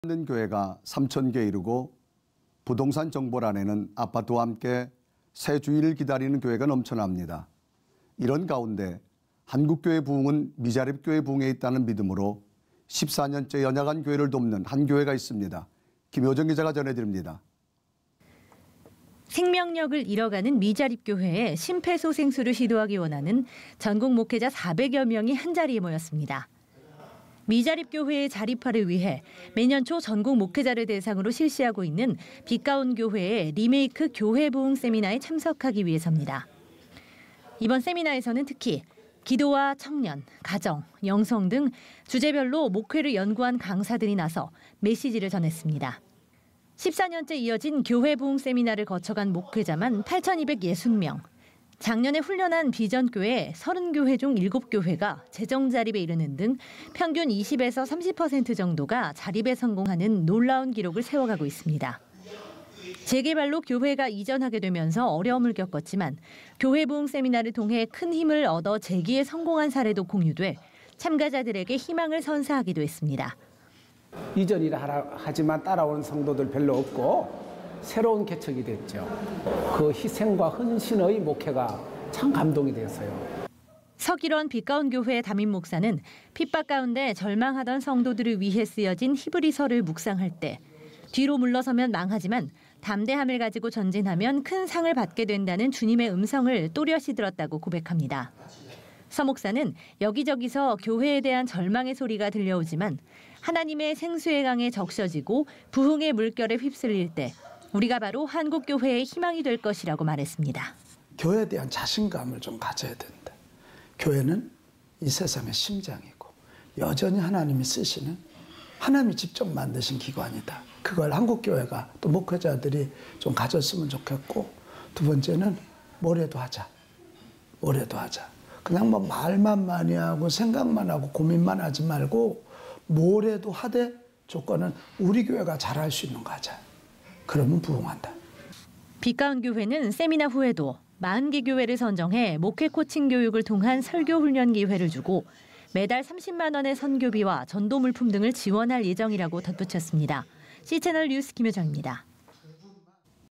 ‘다시 뛰자, 한국교회’ 교회부흥세미나 성료
한 해에 문 닫는 교회가 3천개에 이르고, 부동산 정보란에는  새 주인을 기다리는 교회가 넘쳐납니다. 14년째 연약한 교회를 돕는 한 교회의 사역 현장을 김효정 기자가 취재했습니다.

생명력을 잃어가는 미자립교회에 심폐소생술을 시도하기 원하는 전국 목회자 4백여 명이 한자리에 모였습니다.

미자립교회의 자립화를 위해 매년 초 전국 목회자를 대상으로 실시하고 있는 빛가온교회의 리메이크 교회부흥세미나에 참석하기 위해섭니다.

이번 세미나에서는 특히 기도와 청년, 가정, 영성 등 주제별로 목회를 연구한 강사들이 나서 메시지를 전했습니다.

14년째 이어진 교회부흥세미나를 거쳐 간 목회자만 8260명, 작년에 훈련한 비전교회 30교회 중 7교회가 재정 자립에 이르는 등 평균 20~30% 정도가 자립에 성공하는 놀라운 기록을 세워가고 있습니다.

재개발로 교회가 이전하게 되면서 어려움을 겪었지만 교회부흥세미나를 통해 큰 힘을 얻어 재기에 성공한 사례도 공유돼 참가자들에게 희망을 선사하기도 했습니다.

INT 정연석 목사 / 마곡전동교회
이전이라고 하지만 따라 오는 성도들 별로 없고 새로운 개척이 됐죠 (서길원 목사님의) 희생과 헌신의 목회가 참 감동이 됐습니다

서길원 빛가온교회 목사는 핍박 가운데 절망하던 성도들을 위해 쓰여진 히브리서를 묵상할 때 ‘뒤로 물러서면 망하지만 담대함을 가지고 전진하면 큰 상을 받게 된다’는 주님의 음성을 또렷이 들었다고 고백합니다.

서 목사는 여기저기서 교회에 대한 절망의 소리가 들려오지만 하나님의 생수의 강에 적셔지고 부흥의 물결에 휩쓸릴 때 우리가 바로 한국교회의 희망이 될 것이라고 말했습니다.

INT 서길원 목사 / 빛가온교회
교회에 대한 자신감을 가져야 한다 교회는 이 세상의 심장이고 여전히 하나님이 쓰시는 하나님이 직접 만드신 기관이다 그것을 한국교회가, 그리고 목회자들이 가졌으면 좋겠고 두 번째는 뭐라도 하자 말만 많이 하고 생각하고 공부만 하지 말고 뭐라도 하되 조건은 우리교회가 잘 할 수 있는 것을 하자 그러면 부흥한다

빛가온교회는 세미나 후에도 40개 교회를 선정해 목회코칭교육을 통한 설교 훈련 기회를 주고, 매달 30만원의 선교비와 전도물품 등을 지원할 예정이라고 밝혔습니다. C채널뉴스, 김효정입니다.